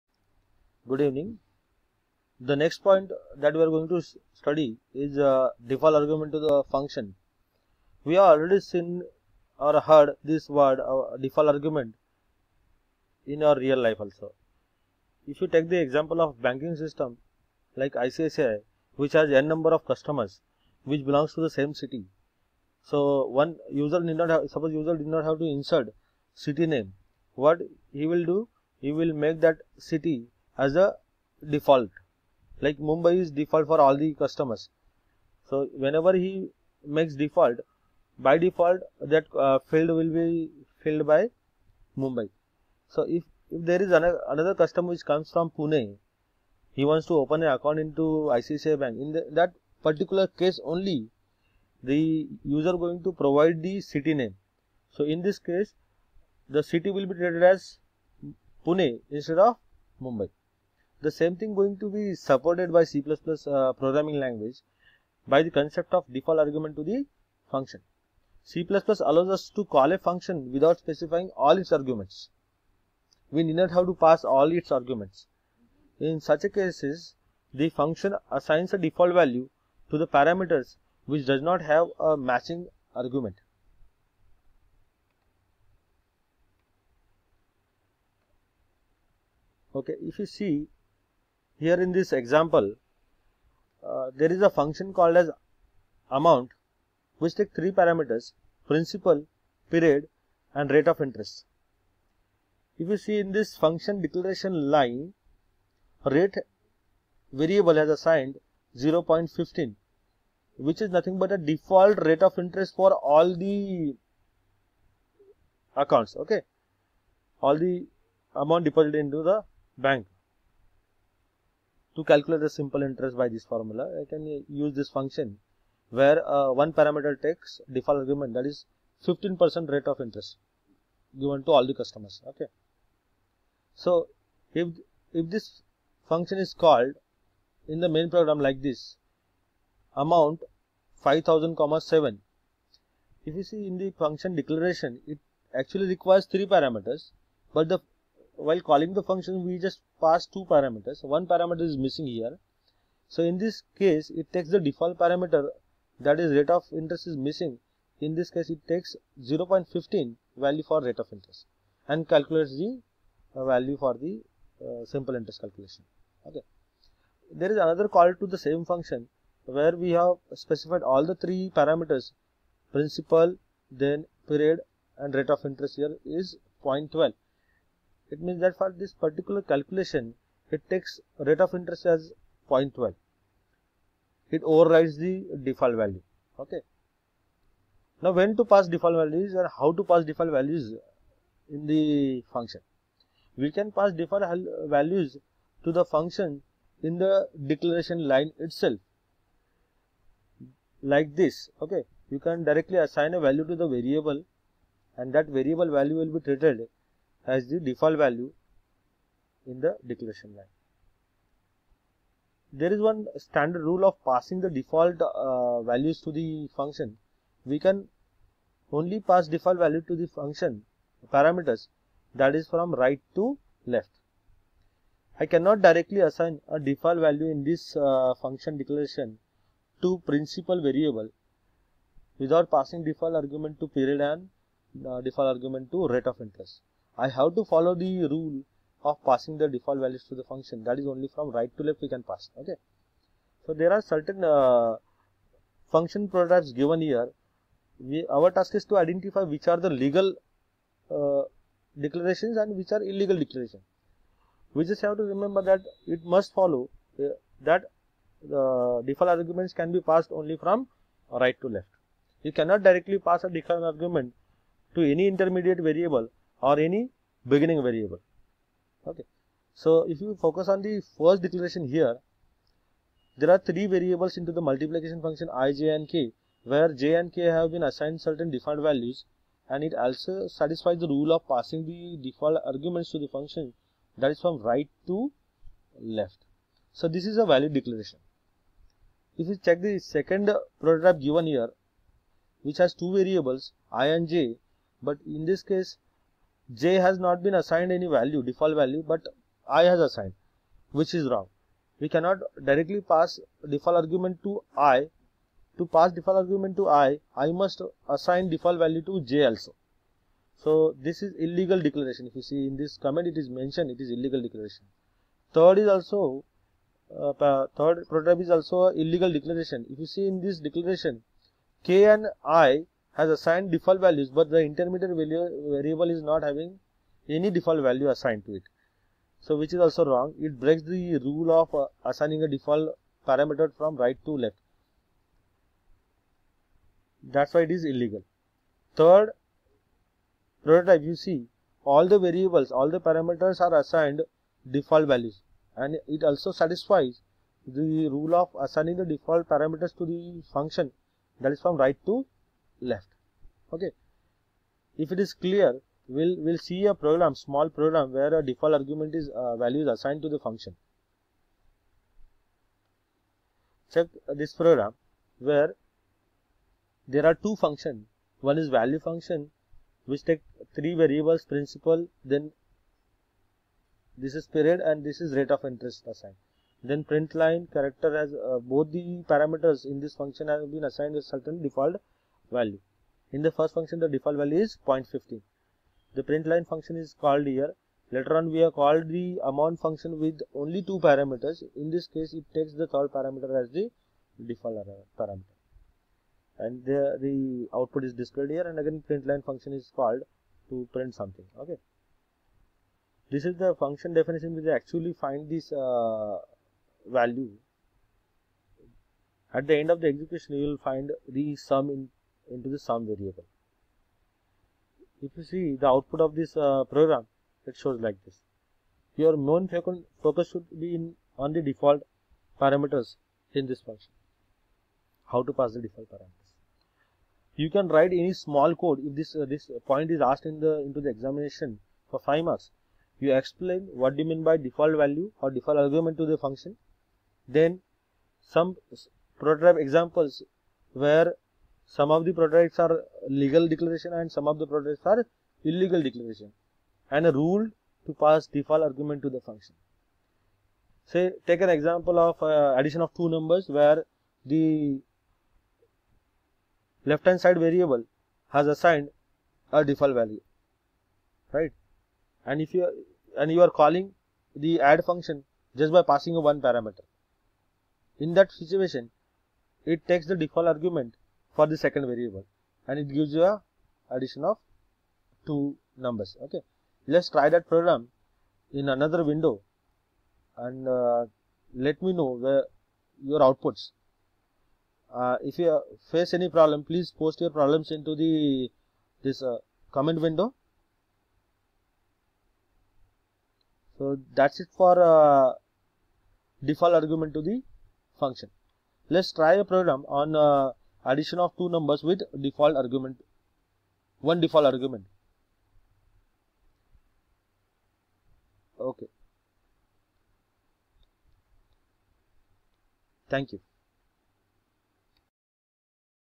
Default Argument
This video consist of Default Argument to a function in C++